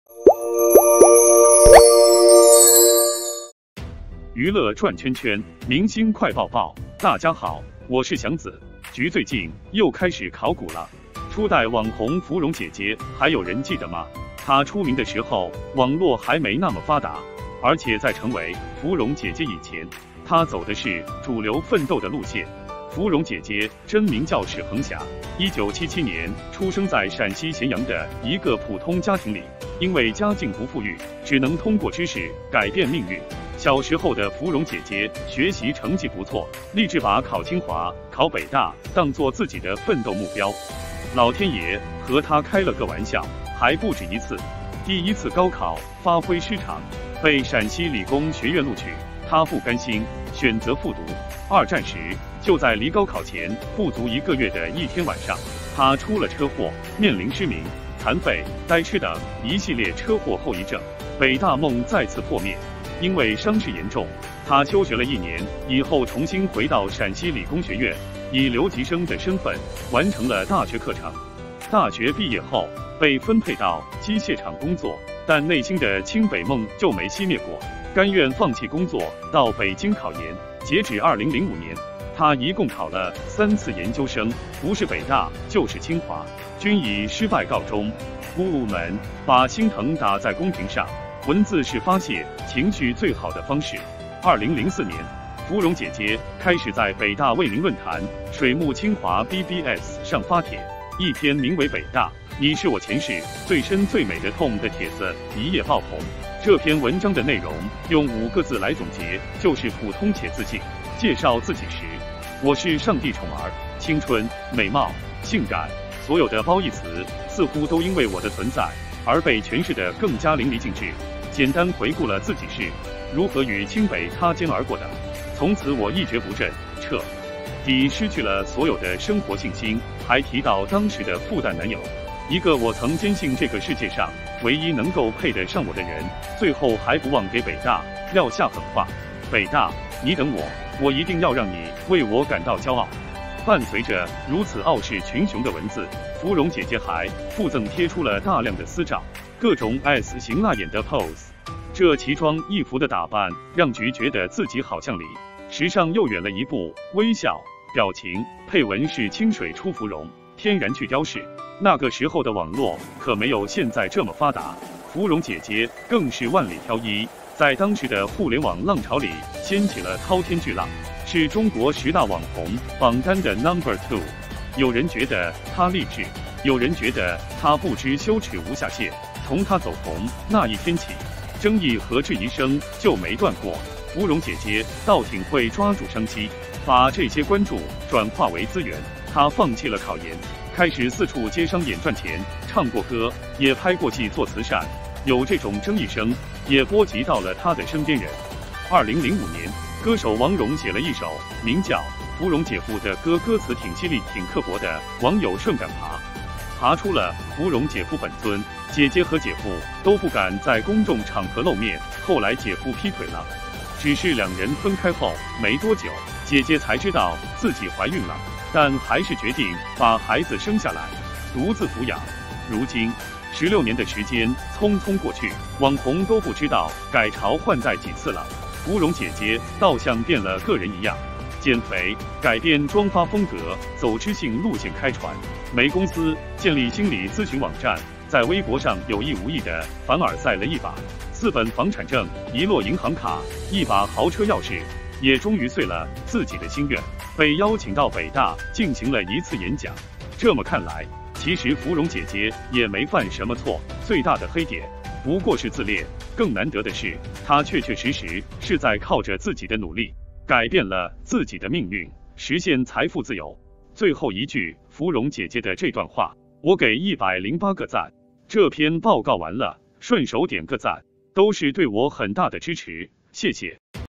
你还记得第一代网络明星芙蓉姐姐吗？她现在是一家媒体公司的老板？
橘最近又开始考古了 ?初代网红“芙蓉姐姐”还有人记得吗？她出名的时候网络还没那么发达。而且在成为“芙蓉姐姐”以前，她走的是主流奋斗的路线。